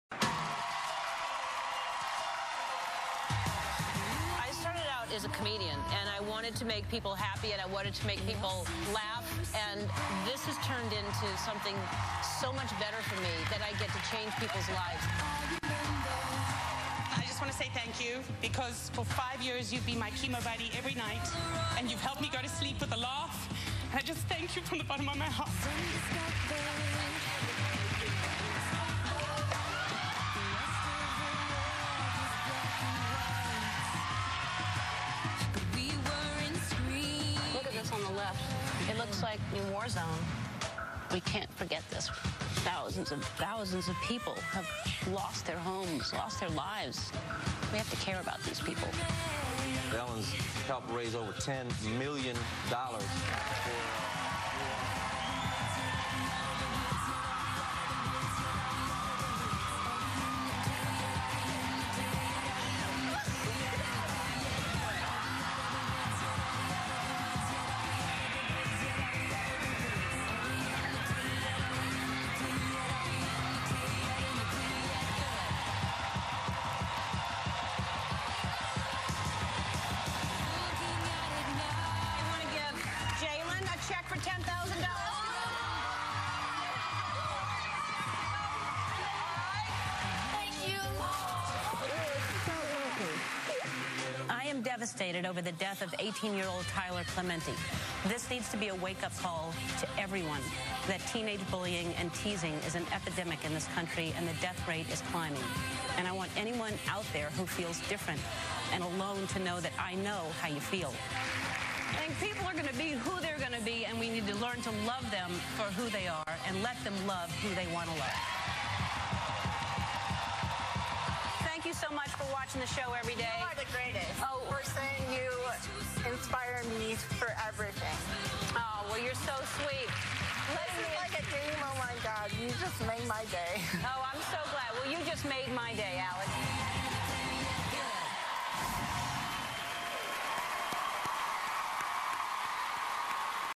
Flashback📺Ellen DeGeneres is a human treasure
Ellen DeGeneres is one of the biggest gift to our planet by God this woman is inspiring, kind, humble, supportive, loving, respectful, caring and friendly.
Yes she got it all. She's an Angel.